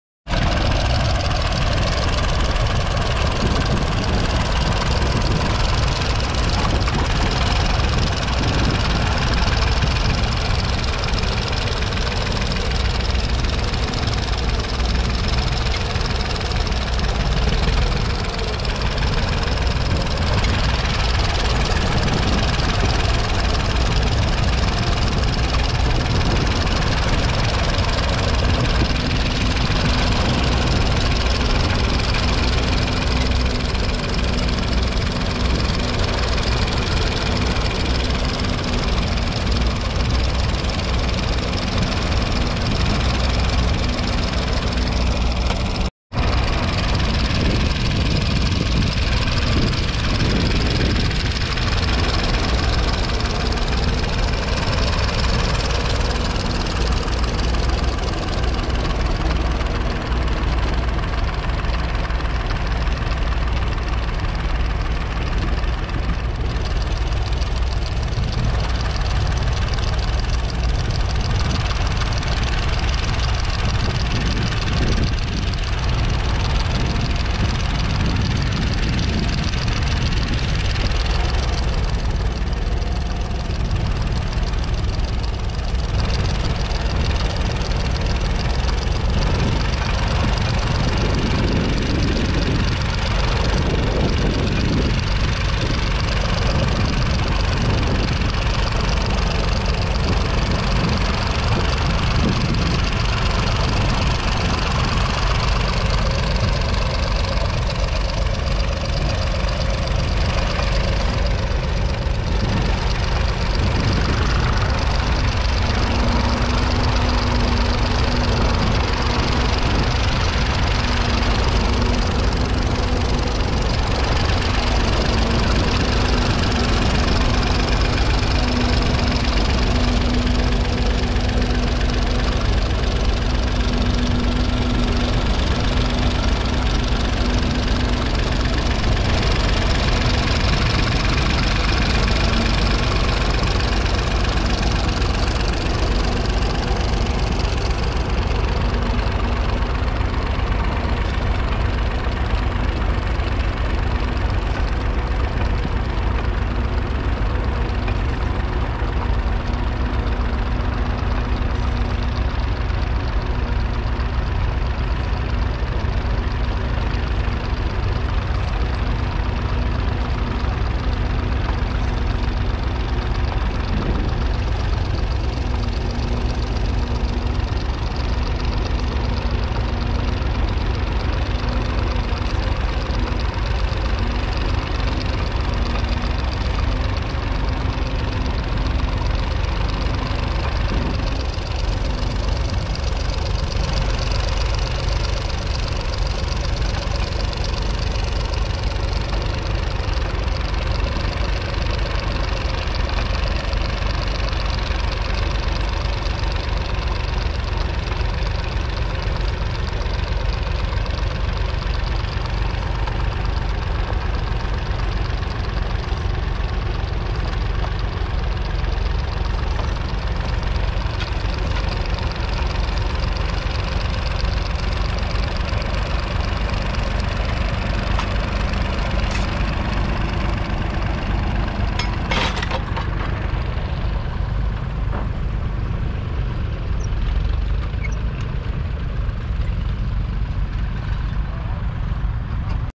1959 Nuffield Universal 3 2.6 Litre 3-Cyl Diesel Tractor (34HP) with Ransomes Plough
1959 Nuffield Universal 3  2.6 Litre 3-Cyl Diesel Tractor (34HP) ..
Ransomes 2 Furrow Trailed Plough

Hereford Ploughing Match - Hereford, England, United Kingdom - Saturday 17th April 2021